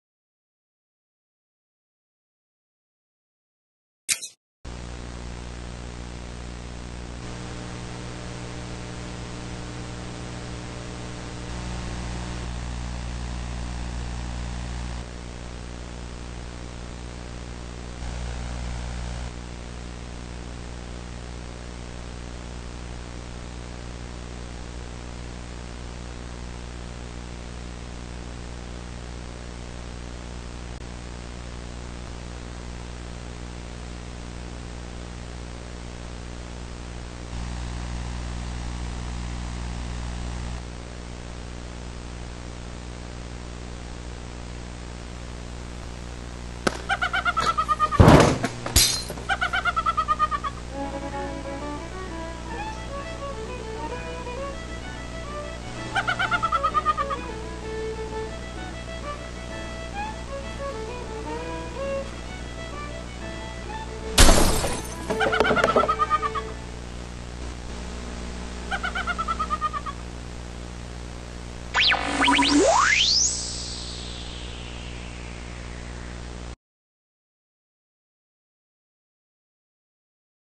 Real Live Leprechaun Caught on Video
We made Leprechaun Traps and set out to catch one - what we caught on video was WAY BETTER